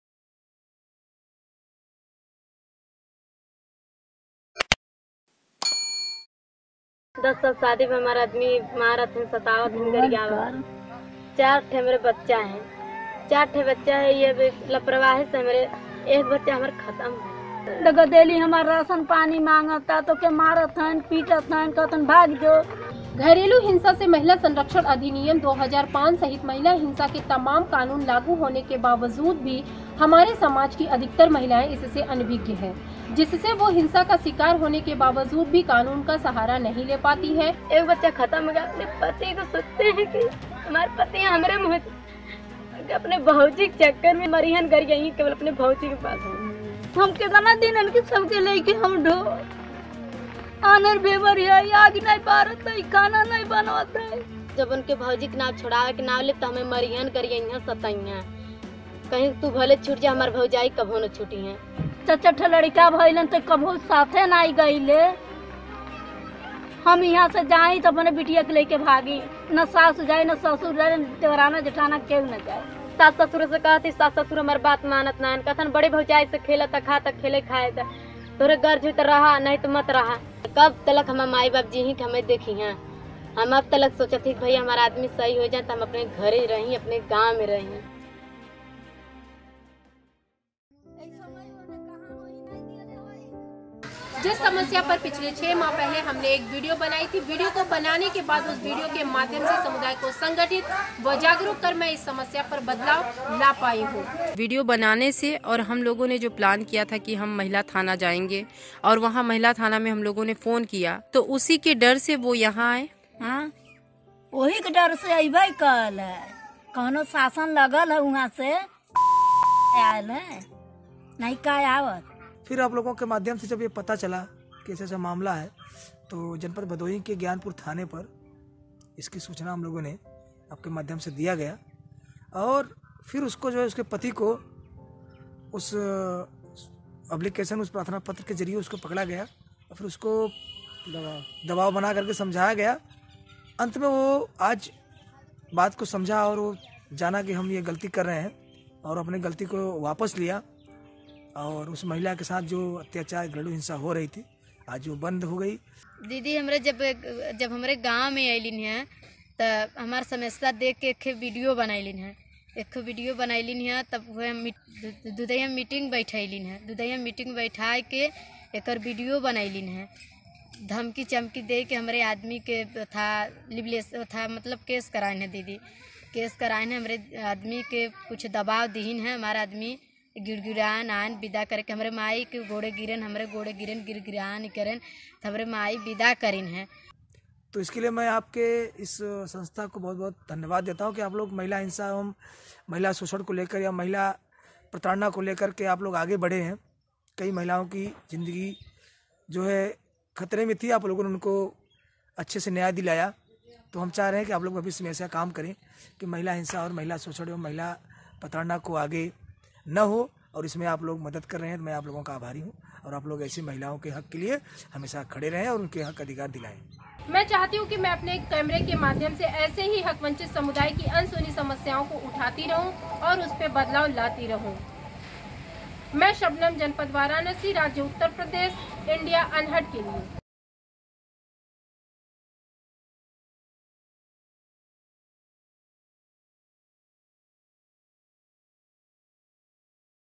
IMPACT | शबनम के पहल से आया बड़ा बदलाव, अनीता के साथ हो रहे घरेलु हिंसा पर लगा लगाम
महिला थाना जाकर साथ ही फ़ोन पर बात चित कर थाना अधिकारी को अवगत कराया साथ ही साथ समुदाय में घरेलु हिंसा के इस मुद्दे पर चर्चा की| अनीता के पति पर दवाब बना पुलिस ने भी उससे बात चित की और दवाब बनाया जिसके बाद आज अनीता पर हो रहे घरेलु हिंसा पर लगाम लग गया है|

Community Correspondent Shabnam Begum reports from Varanasi, Uttar Pradesh for Video Volunteers.  Learn more about Shabnam and see other videos of hers